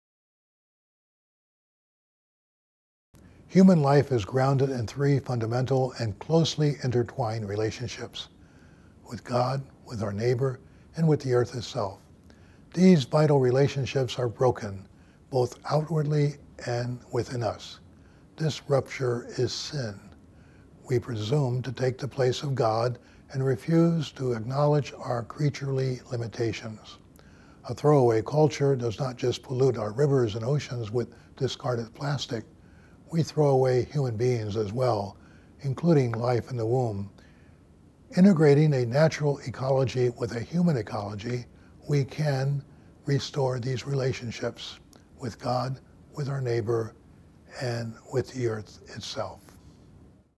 Conversations with Archbishop Thomas Wenski - Natural and Human Ecology
Archbishop Thomas Wenski is one of the Sun-Sentinel's South Florida 100, community leaders who are asked to submit two "mini op-eds" each week for publication in the newspaper. The archbishop has begun to record those weekly op-eds, and they will now be posted on the archdiocesan website and social media.